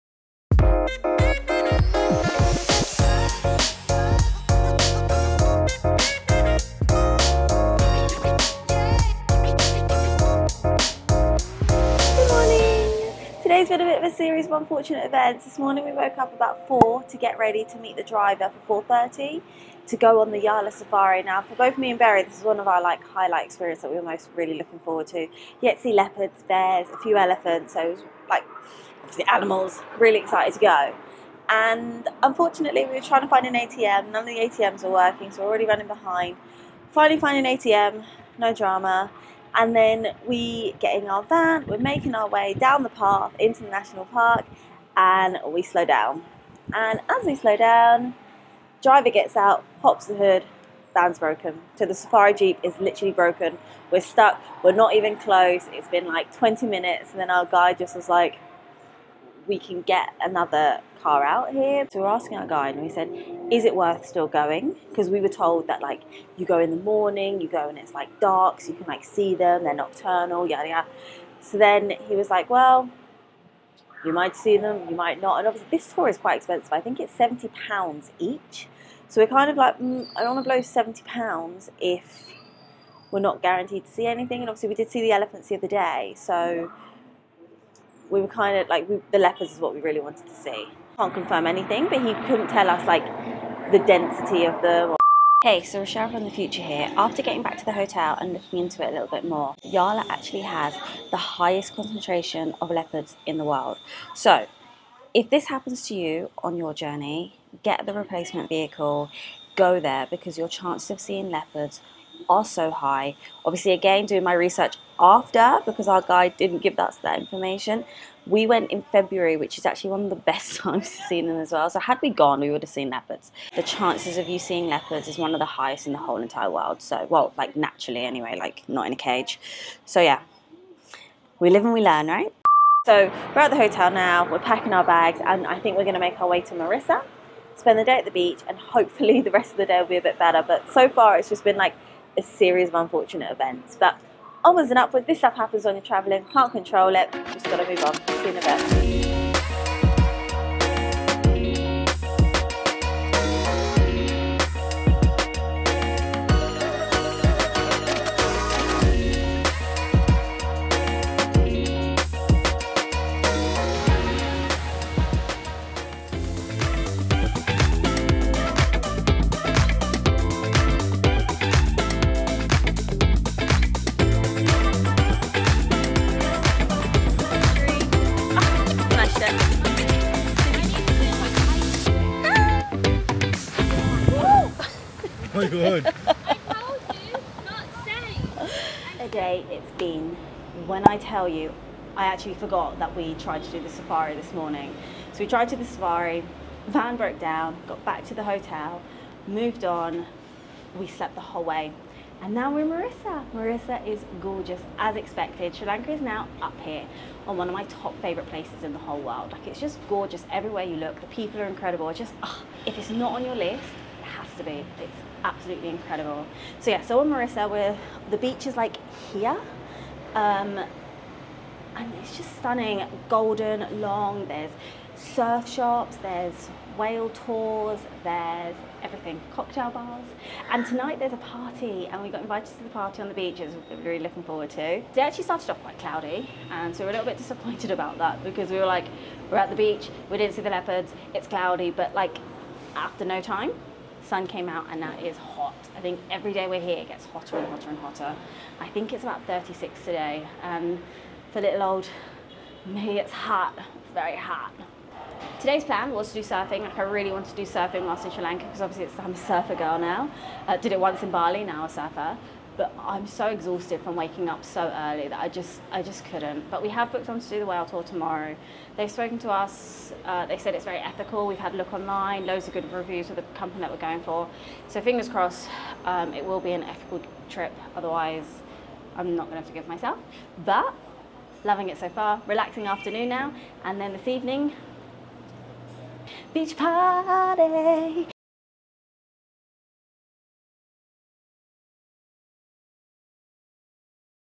Unforeseen Crisis In Yala National Park | Sri Travel Vlog
Join us on a rollercoaster of emotions and unexpected adventures in our latest Sri Lanka travel vlog 🌴🚐. Our plan was to explore the breathtaking Yala National Park, and see the leopards but fate had other plans. Just when we thought our day couldn't get any more eventful, our van decided this trip wasn't for us! But, we ended up spending an unforgettable day at the serene Mirissa beach, soaking up the sun, and making memories that will last a lifetime. 🌊☀️

Despite the change of plans, we discovered the beauty of embracing the unexpected and finding joy in the moments that catch us off guard. From the breakdown to the beach, this vlog is a testament to the unpredictable nature of travel and the incredible stories that come from it. Your support means the world to us! 🌍💖